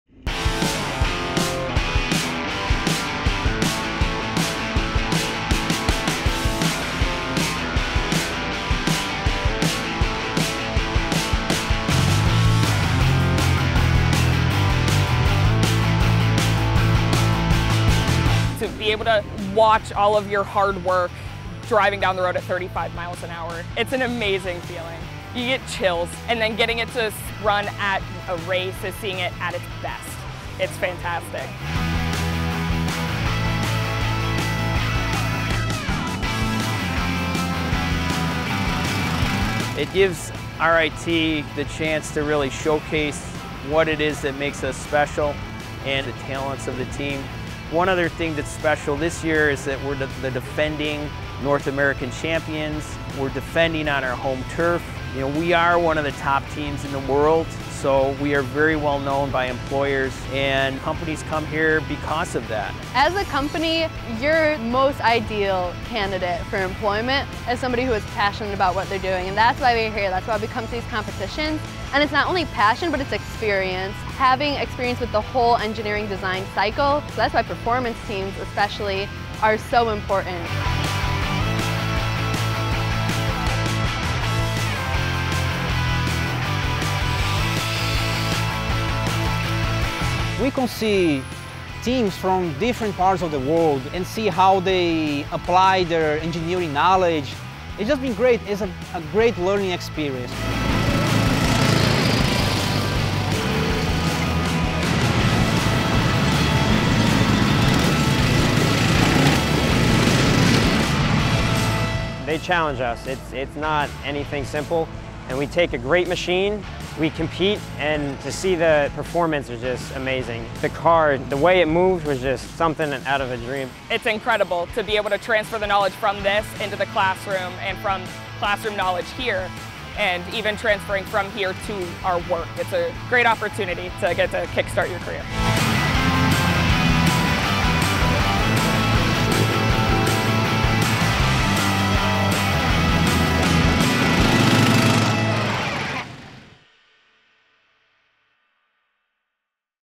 Baja SAE Rochester World Challenge 2019
Teams representing 100 colleges from six countries competed at the 2019 Baja SAE World Challenge hosted by RIT on June 6-9. The teams competed in multiple events with off-road race cars they designed and built, culminating in a four-hour endurance race.